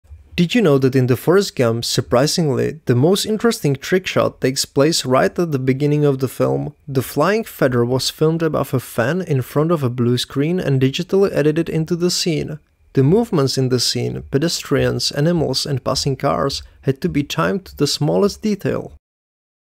Did you know that in Forrest Gump greatest movie effect was...
Did you know that in the Forrest Gump in the scene with Forrest playing table tenis there is something strange about it? Table tenis in the movie wasn‘t played with a real ball, the ball was added later by computer animation so that it hit the players' bats precisely.